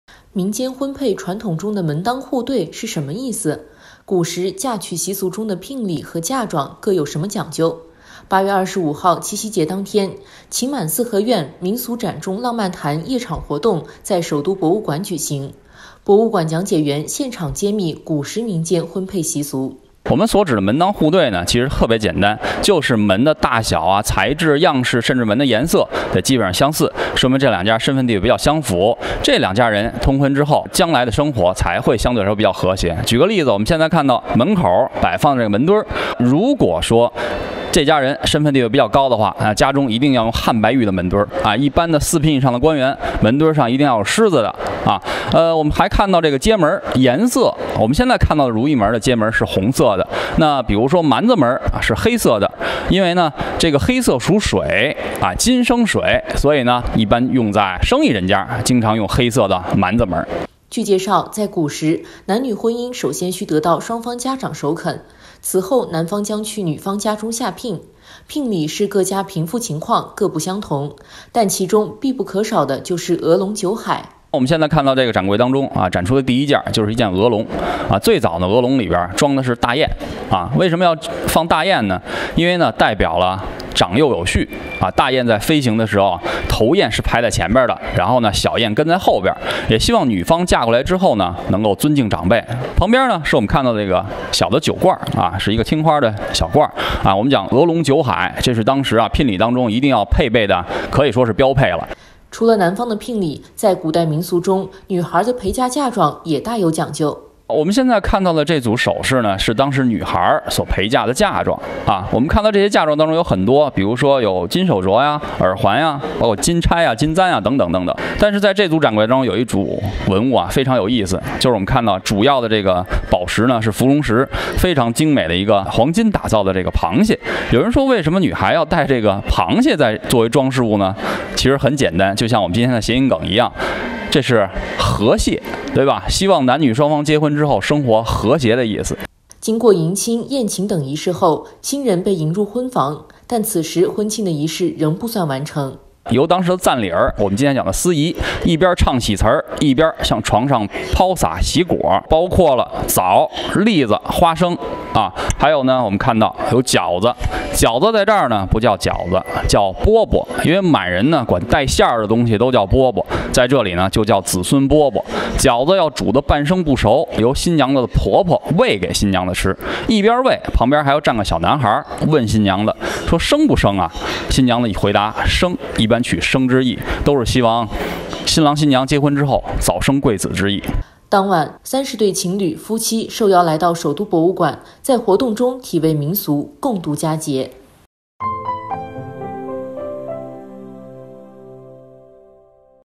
鹅笼酒海、门当户对 这些古代婚配民俗你知道吗？
民间婚配传统中的“门当户对”是什么意思？古时嫁娶习俗中的聘礼和嫁妆各有什么讲究？8月25日“七夕节”当天，“情满四合院，民俗展中浪漫谈”夜场活动在首都博物馆举行。博物馆讲解员现场揭秘古时民间婚配习俗。